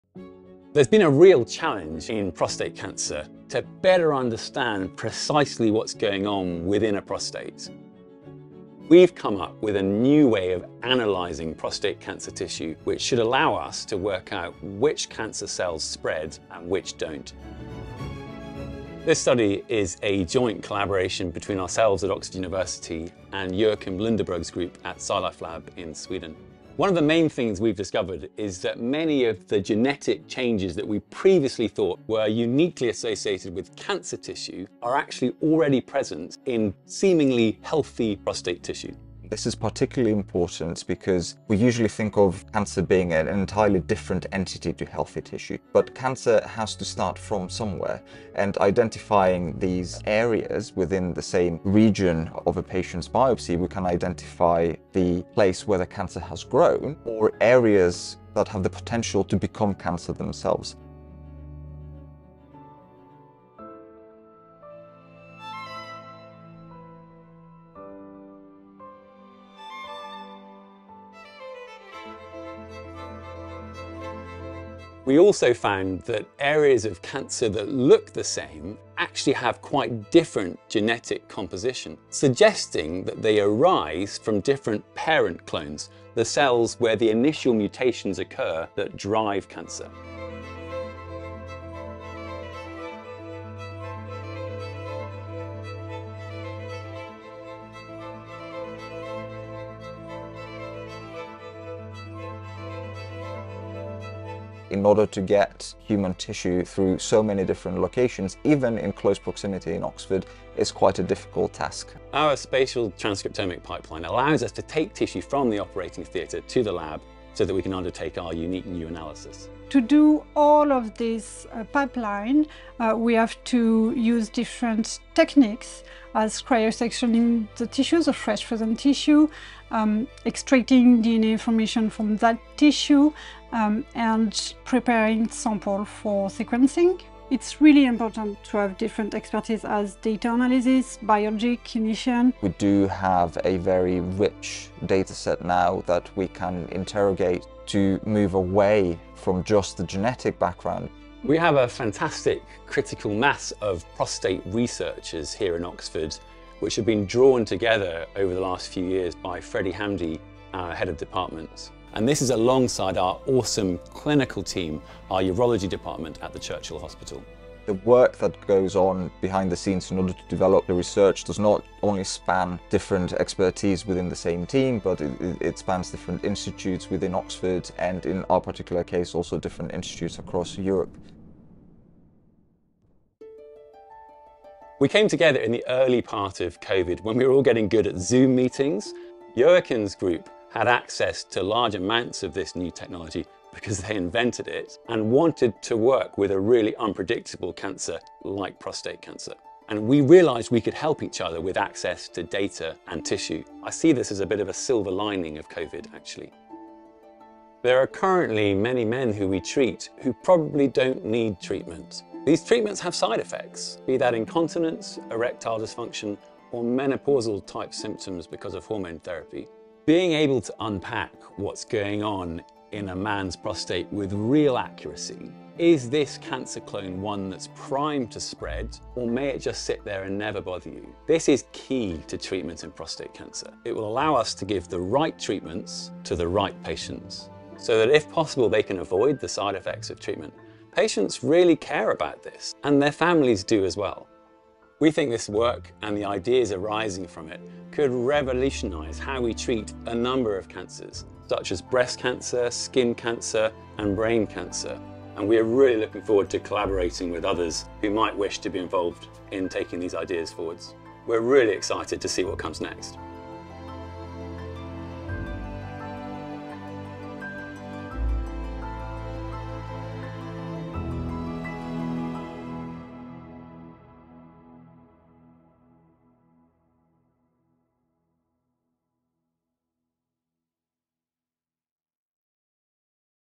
Genetic mapping of tumours: New breakthroughs in prostate cancer
Alastair Lamb, Sandy Figiel, Dimitrios Doultsinos and colleagues from the Nuffield Department of Surgical Sciences (NDS) Prostate Biology Group discuss the findings of a recent paper published in Nature, the spatial prostate biology programme in Oxford, the potential for future collaborations and the hope that these findings will improve care of patients with prostate cancer.

Featuring: Andrew Erickson, Mengxiao Xi, Joakim Lundeberg, Ian Mills, Dan Woodcock, Wencheng Yin, and Ninu Poulose.

Supported by Oxford’s excellent Department of Urology based at the Churchill Hospital and Pathology at the John Radcliffe Hospital.